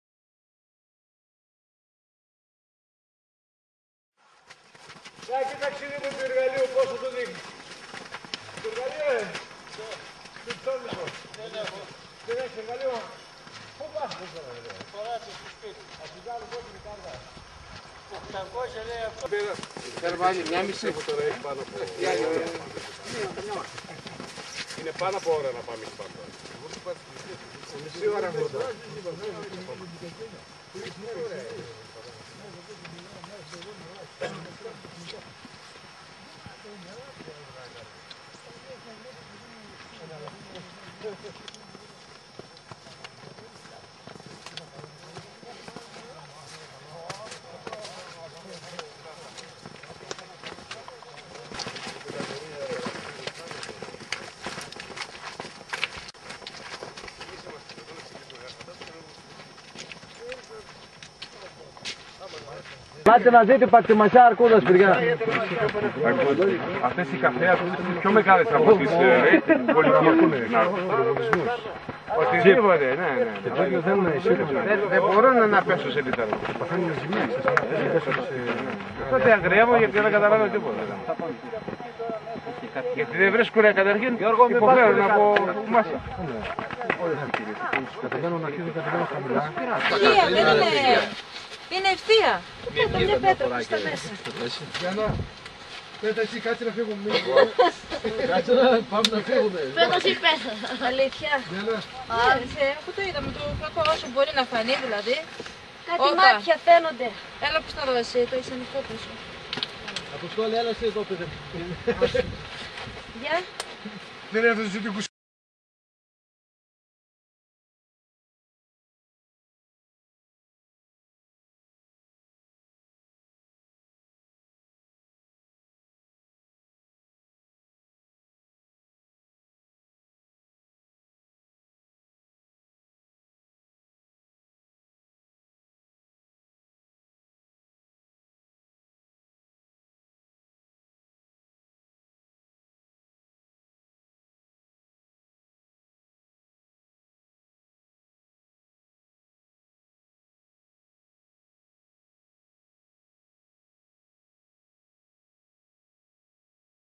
ΚΑΚΟΠΛΕΥΡΙ
Η ανάβαση στην κορυφή Πέτρα Μπισθάνι με εκκίνηση από το Κακοπλεύρι έγινε από τα μέλη του Συλλόγου Πεζοπορίας Ορειβασίας Τρικάλων στις 16/12/2012